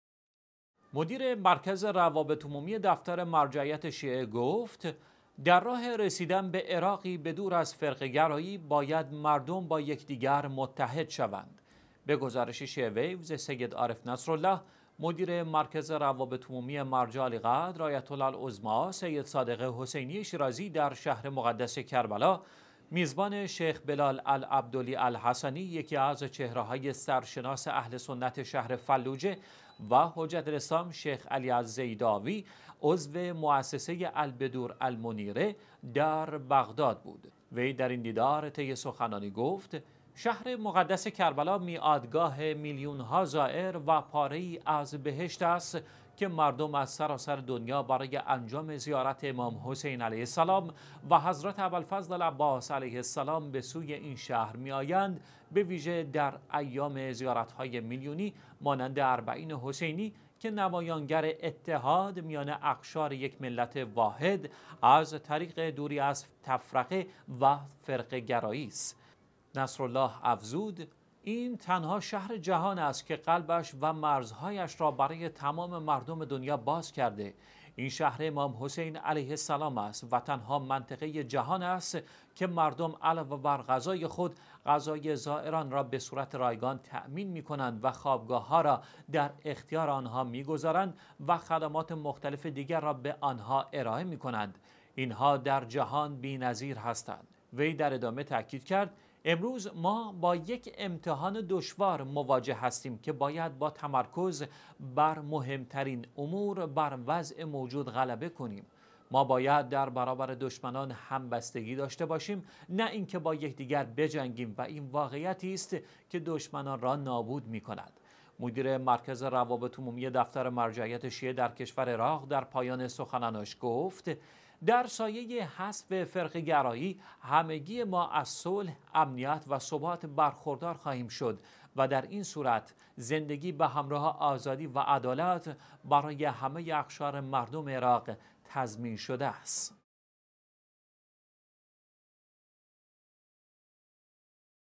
تاکید مدیر مرکز روابط عمومی دفتر مرجعیت، بر اتحاد عراقی ها به دور از فرقه گرایی
مدیر مرکز روابط عمومی دفتر مرجعیت شیعه گفت: در راه رسیدن به عراقی به دور از فرقه گرایی، باید مردم با یکدیگر متحد شوند.
به گزارش شیعه ویوز، «سید عارف نصر الله» مدیر مرکز روابط عمومی دفتر مرجع عالی قدر «آیت الله العظمی سید صادق حسینی شیرازی» در شهر مقدس کربلا،  میزبان «شیخ بلال العبدلی الحسنی» یکی از چهره های سرشناس اهل سنت شهر «فلوجه» و «حجت الاسلام شیخ علی الزیداوی» عضو موسسه «البدور المنیرة» در بغداد بود.
وی در این دیدار طی سخنانی گفت:  شهر مقدس کربلا، میعادگاه میلیون ها زائر، و پاره ای از بهشت است که مردم از سراسر دنیا برای انجام زیارت امام حسین علیه السلام و حضرت اباالفضل العباس علیه السلام به سوی این شهر می آیند، به ویژه در ایام زیارت های میلیونی مانند اربعین حسینی، که نمایان گر اتحاد میان اقشار یک ملت واحد از طریق دوری از تفرقه و فرقه گرایی است.
نصر الله افزود: این تنها شهر جهان است که قلبش و مرزهایش را برای تمام مردم دنیا باز کرده، این شهر امام حسین علیه السلام است و تنها منطقه ی جهان است که مردم علاوه بر غذای خود، غذای زائران را به صورت رایگان تأمین می کنند و خوابگاه ها را در اختیار آن ها می گذارند و خدمات مختلف دیگر را به آن ها ارائه می کنند؛  این ها در جهان بی نظیر هستند.
وی در ادامه تاکید کرد:  امروز ما با یک امتحان دشوار مواجه هستیم که باید با تمرکز بر مهم ترین امور، بر وضع موجود غلبه کنیم. ما باید در برابر دشمنان همبستگی داشته باشیم، نه اینکه با یکدیگر بجنگیم و این واقعیتی است که دشمنان را نابود می کند.
مدیر مرکز روابط عمومی دفتر مرجعیت شیعه در کشور عراق، در پایان سخنانش گفت: در سایه ی حذف فرقه گرایی، همگی ما از صلح، امنیت و ثبات برخوردار خواهیم شد و در این صورت، زندگی به همراه آزادی و عدالت برای همه اقشار مردم عراق تضمین شده است.